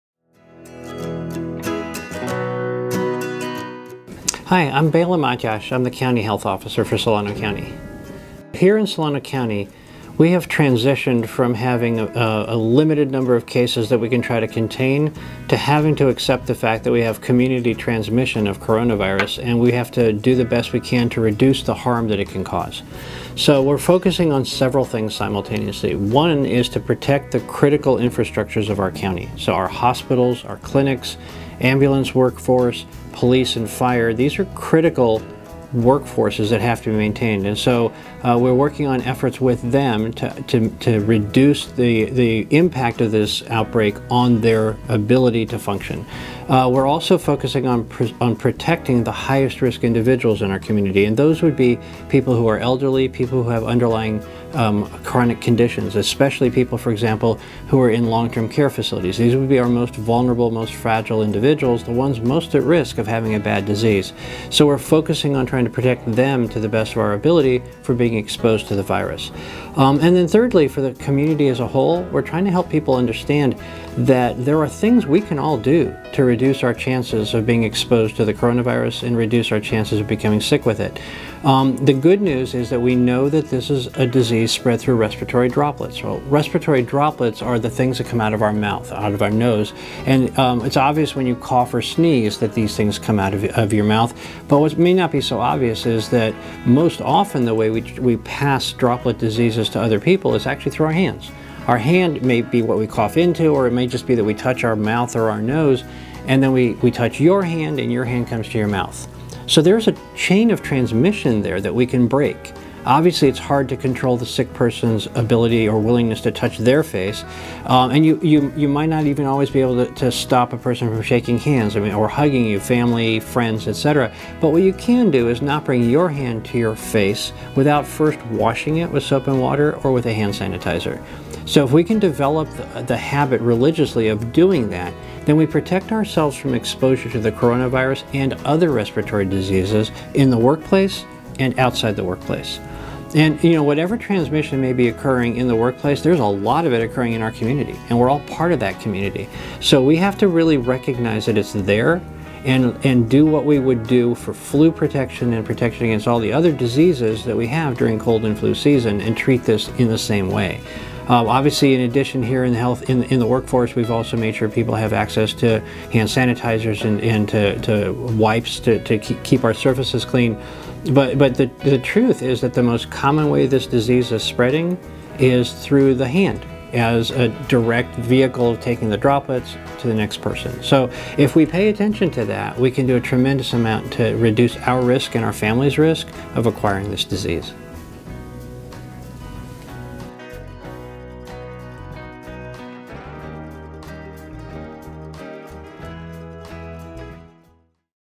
COVID 19 Health Update, with Dr. Bela Matyas, M.D., M.P.H.
A health update on the Coronavirus (COVID-19) and ways to protect yourself, your colleagues and your family during cold and flu season.